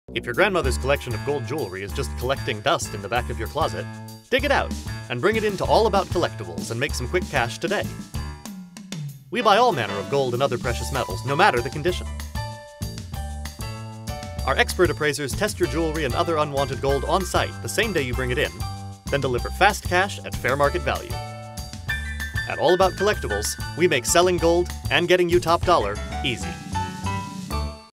All About Collectibles
Tell potential customers about the fast cash they can get when they bring their unwanted gold and jewelry into your business.

"If your grandmother's collection of gold jewelry is just collecting dust in the back of your closet, dig it out and bring it in and make some quick cash today.
We buy all manner of gold and other precious metals, no matter the condition.  Our expert appraisers test your jewelry and other unwanted gold on-site the same day you bring it in, then deliver fast cash at fair market value.
We make selling gold -- and getting you top-dollar -- easy.  "